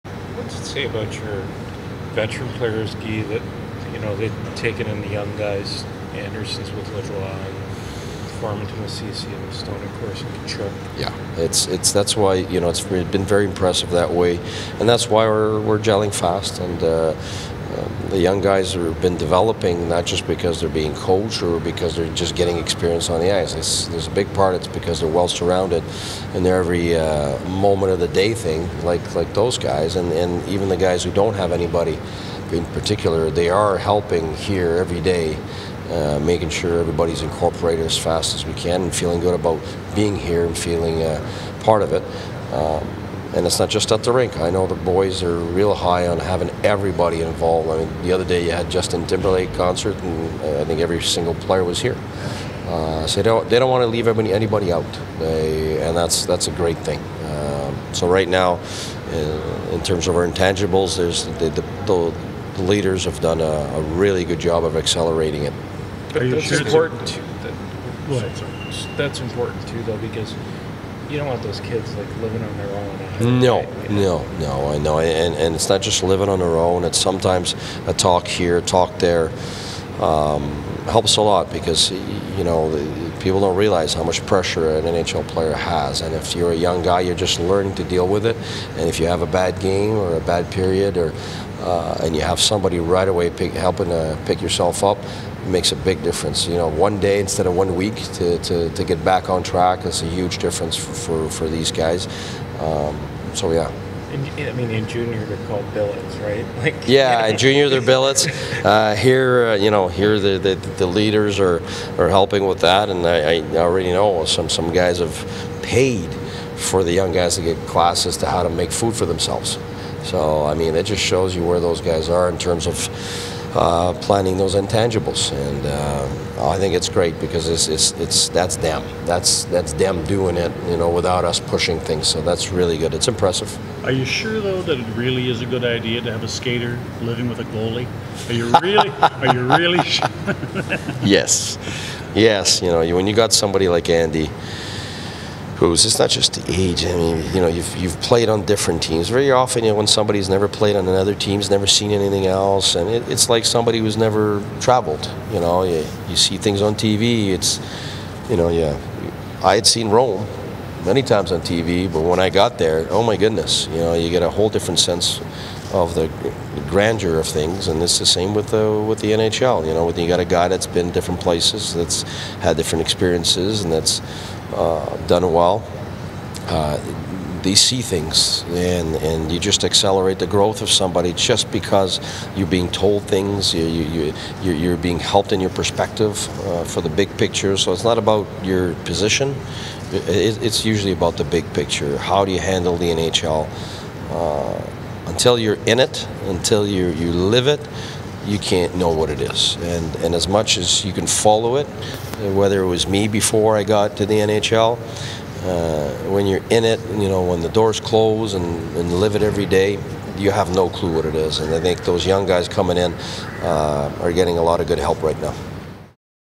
Oct. 14 - Coach Practice Media
Sens head coach Guy Boucher talks about how the veterans are helping the younger players develop off the ice.